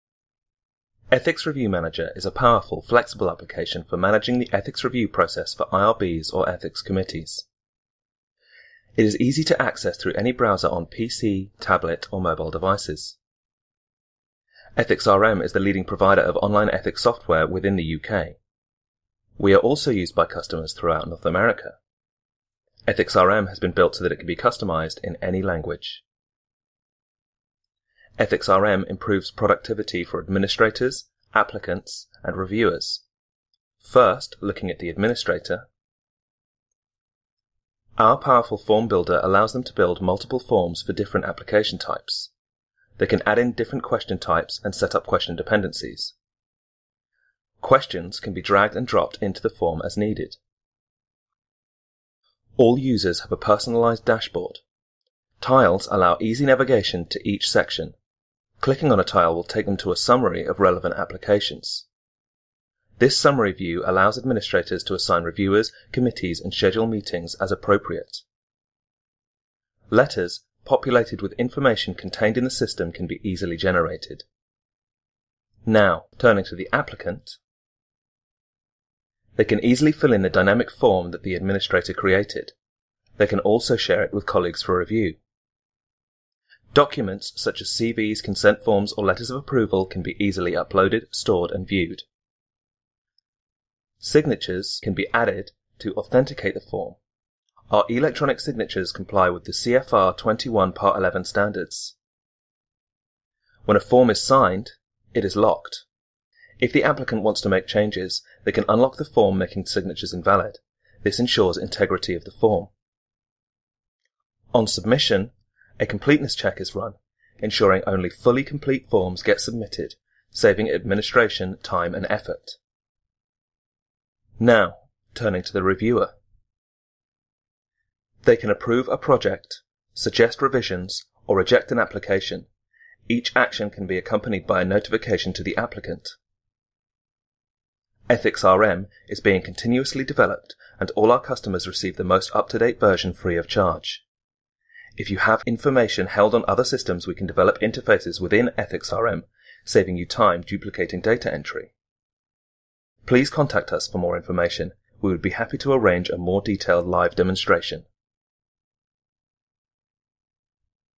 Ethics Review Manager - Overview
Infonetica’s Ethics Review Manager (EthicsRM) solution streamlines the ethics process and delivers significant productivity gains, with the time from application to approval falling by an average of 29%. Better still, through the integrated management, control and communication capacity of Ethics RM you can significantly lower Ethics committee / Institutional Review Board (IRB) administration costs.

Infonetica’s ethics review software is widely used in leading organisations. It is the solution used in the UK’s national ethics clearance systems, IRAS, and has been adopted by regional government and institutions in Australia, New Zealand, Canada and the USA. At the other end of the scale, this adaptable solution is bringing cost and productivity benefits to healthcare and research institutions with a single ethics committee / Institutional Review Board (IRB).

Ethics RM is the latest feature-rich version of Infonetica’s well established ethics management software previously known as RED (Research Database) and Online Forms. Building on 10 years of ethics software experience, Ethics RM is an end-to-end electronic solution that puts configuration of the form and review process entirely in the hands of the user. This vastly reduces implementation cost, time and risk.